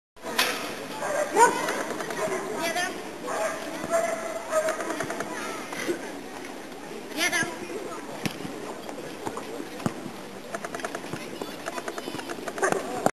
Briard Monika, Russian Ring
Russian Ring Championship of Murmansk area, 1st exercise - obedience (helper have a meat in the hand). Briard Monika 8 years old. 7,5 points from 10. Helper - Vladimir Vetoshkin. Conductor (guid) - Yana Ovchinnikova.